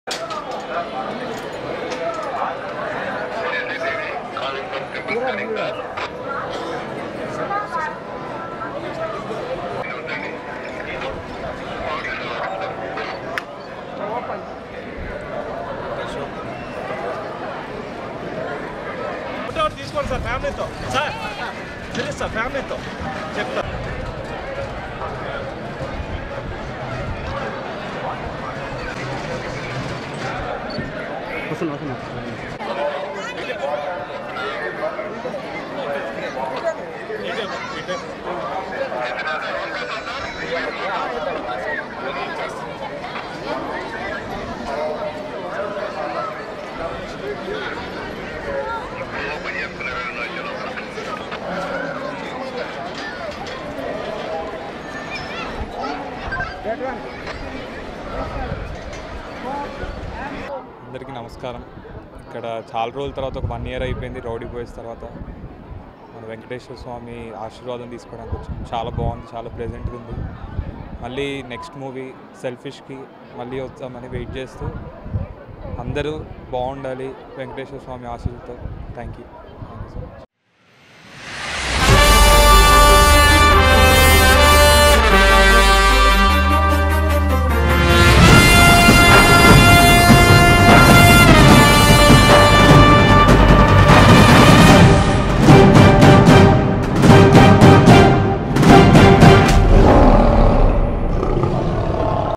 Producer Dil Raju Brother Sirish Reddy Visits Tirumala with his Family | Ashish | LE
Leo Politics

Leo Health

Leo Entertainment

Leo Devotional